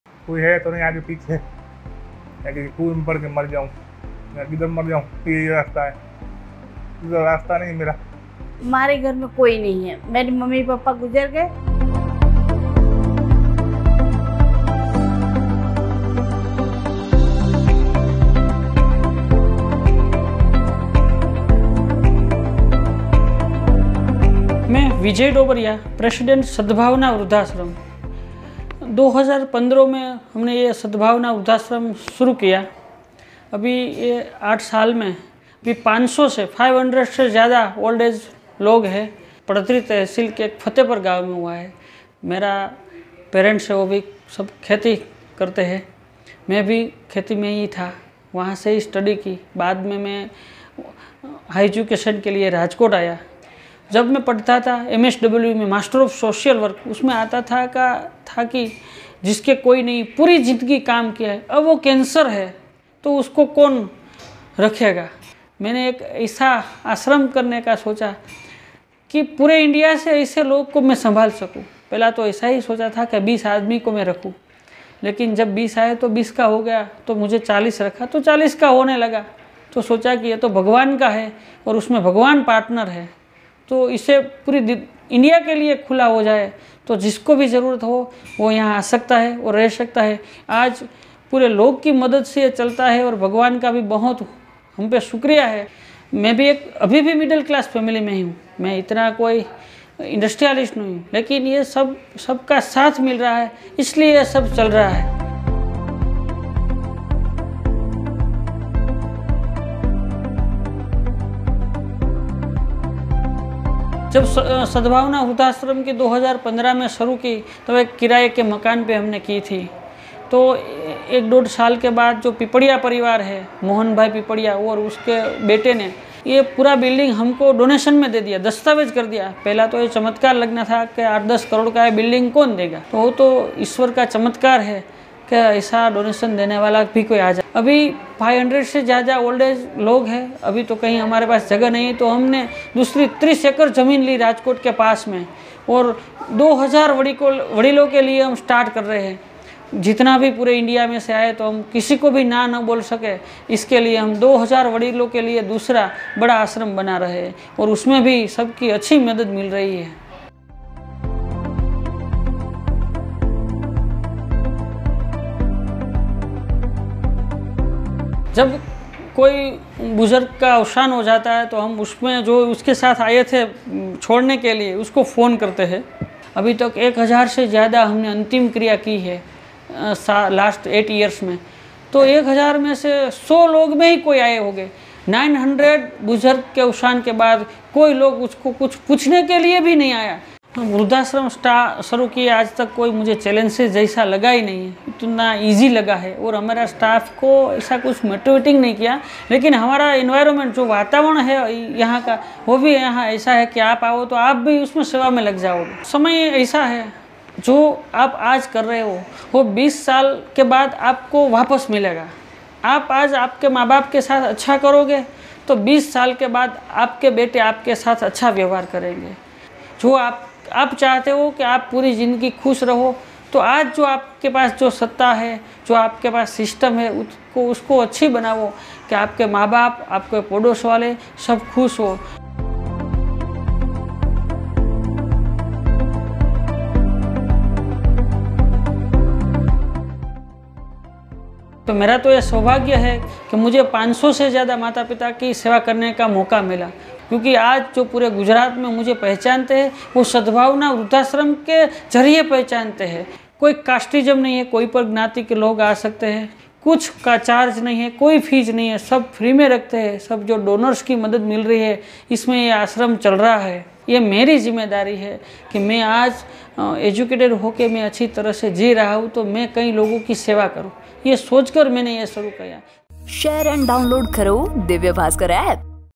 ભાસ્કર ખાસ : જે છોડીને જાય છે તે અંતિમ સંસ્કારમાં પણ આવતા નથી
ભાસ્કર ખાસ : જે છોડીને જાય છે તે અંતિમ સંસ્કારમાં પણ આવતા નથી, જેમનું દુનિયામાં કોઈ નથી તેમના માટે વૃદ્ધાશ્રમ ખોલ્યું; વિનામૂલ્યે રહે છે 500 થી વધુ દાદા-દાદી; રાજકોટના વિજય ડોબરિયાની કહાની